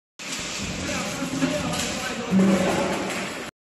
Tunnel fan with air supply distance up to 3000 meters
Tunnel fan with air supply distance up to 3000 meters，Power is up to 132KW! That's really impressive#ventilationfan